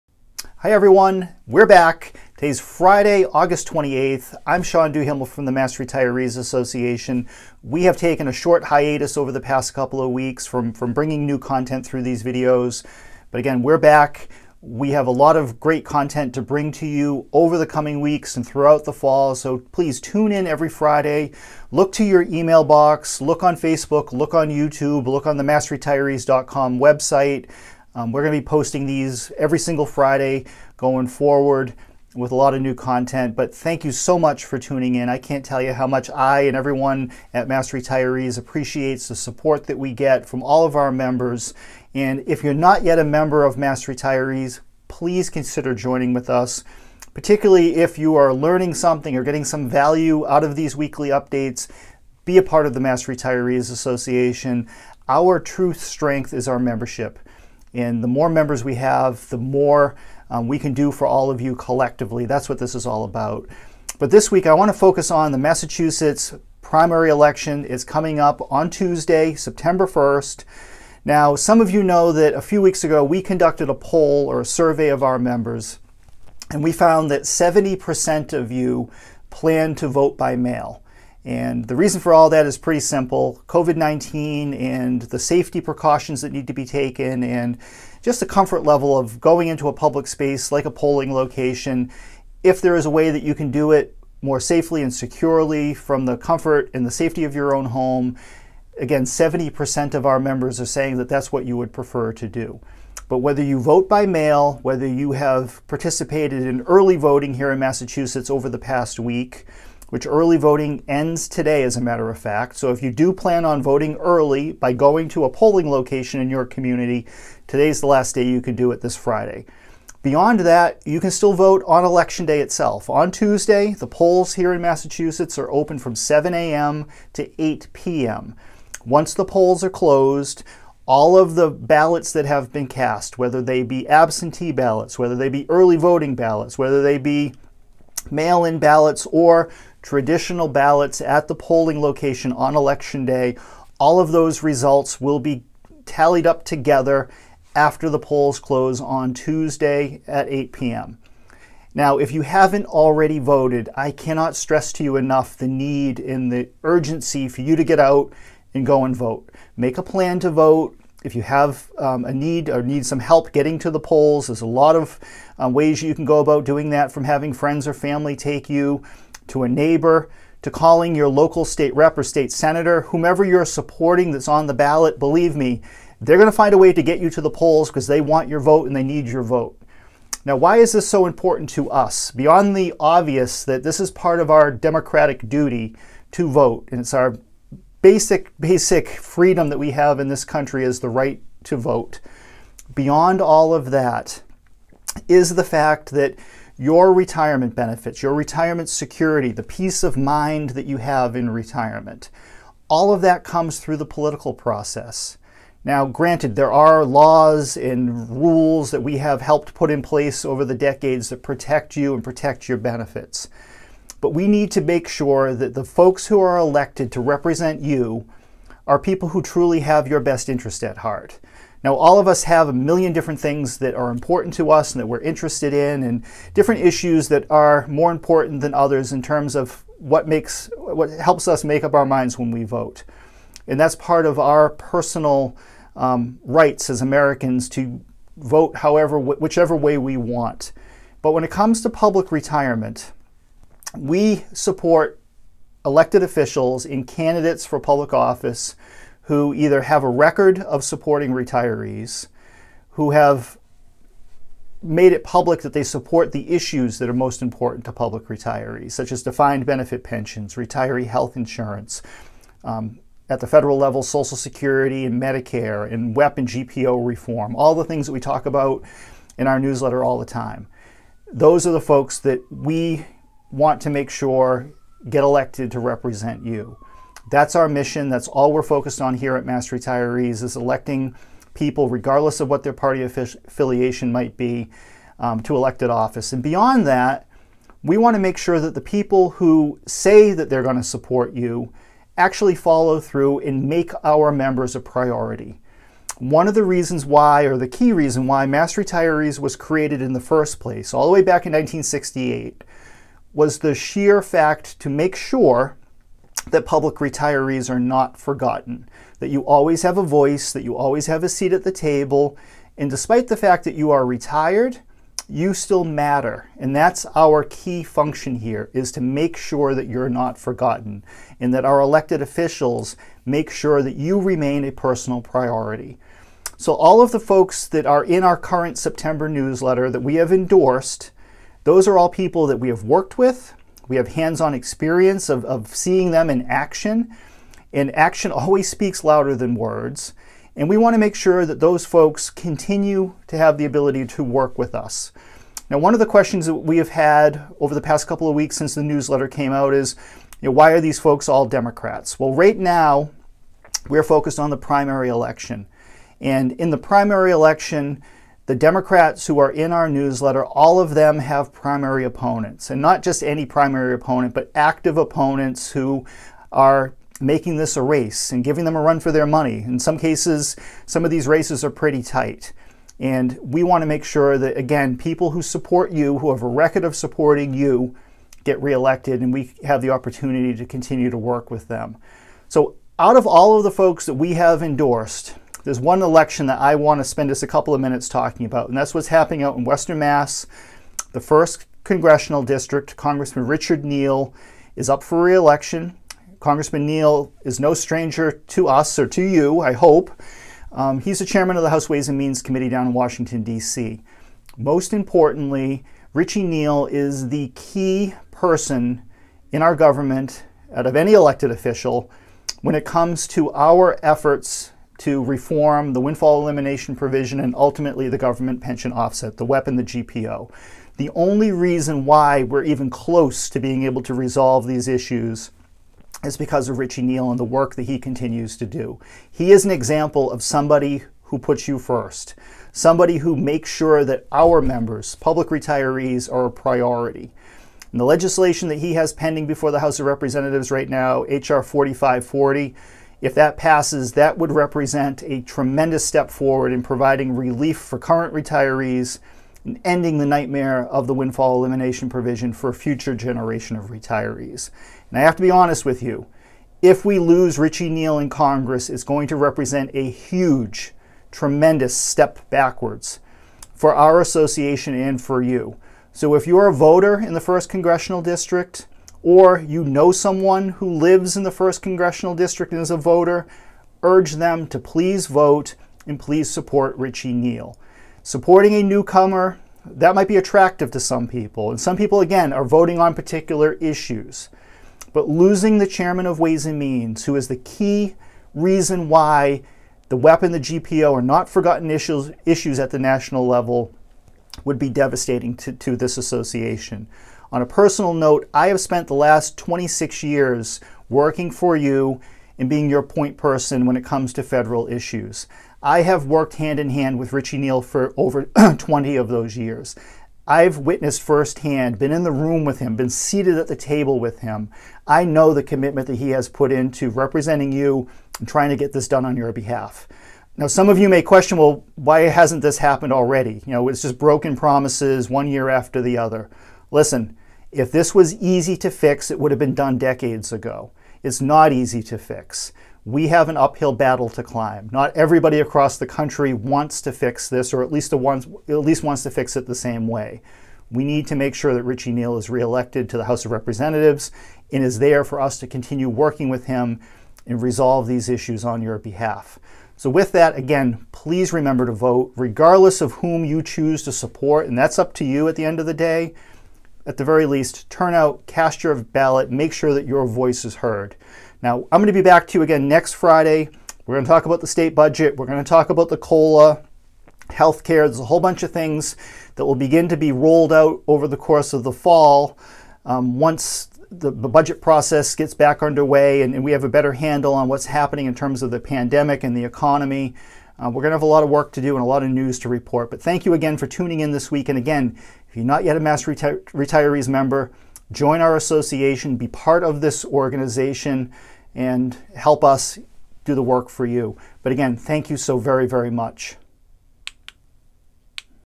Back from hiatus. Go VOTE
Mass Retirees’ weekly updates are back! Tuesday is the Primary Election here in Mass. If you have not yet voted, PLEASE make an effort to get to your polling place and VOTE. We can’t stress strongly enough how important it is to help elect leaders who not only support public retirees, but will also make you a personal priority.